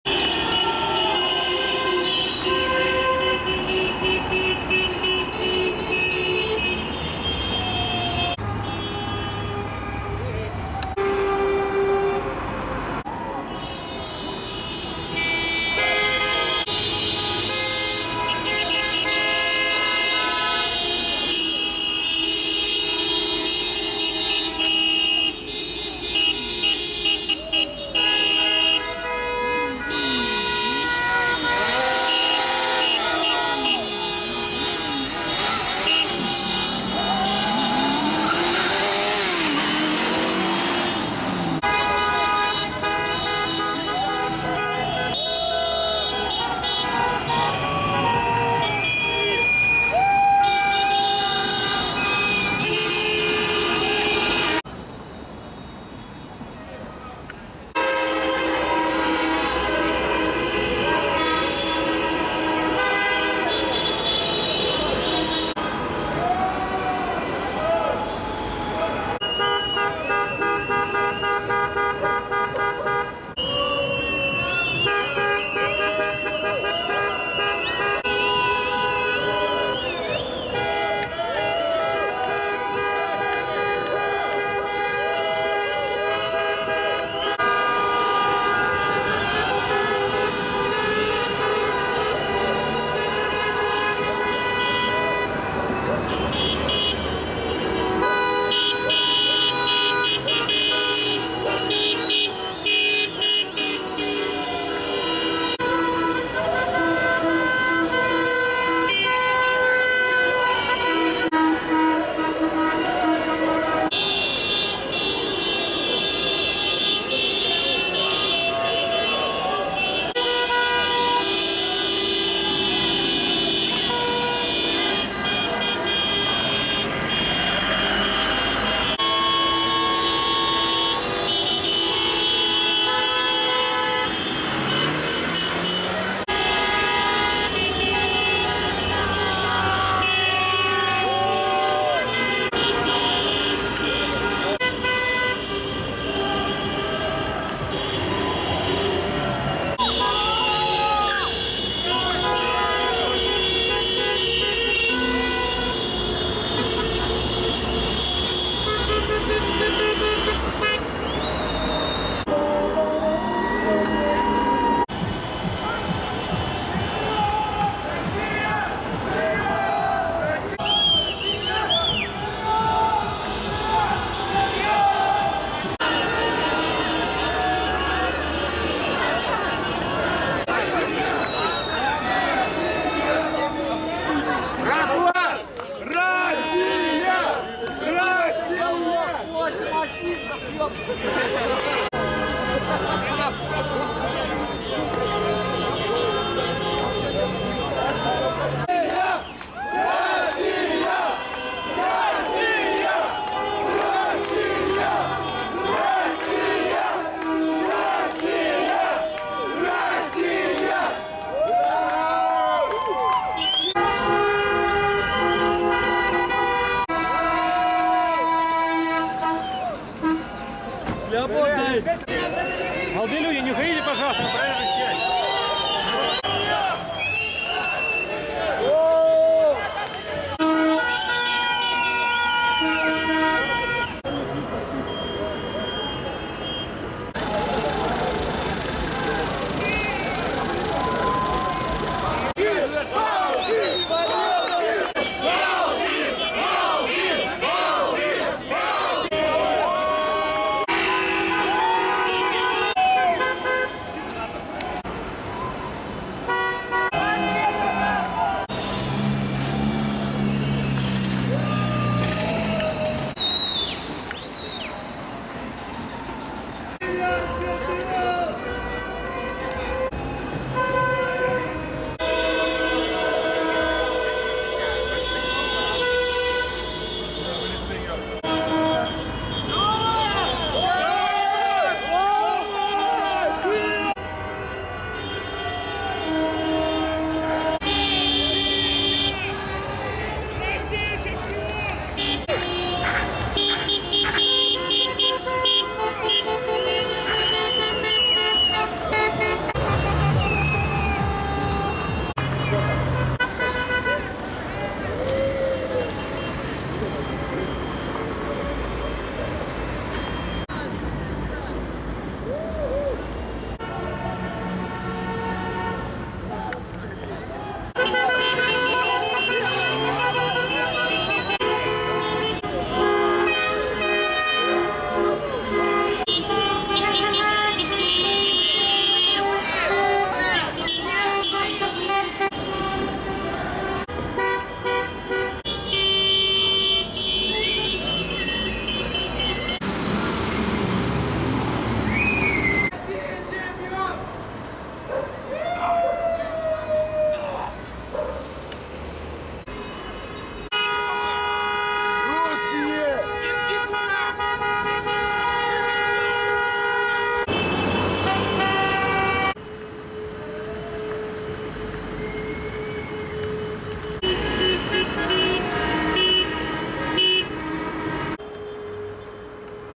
Празднование победы хоккейной сборной России
Жители Петрозаводска празднуют на улицах города победу хоккейной сборной России на Чемпионате мира 2012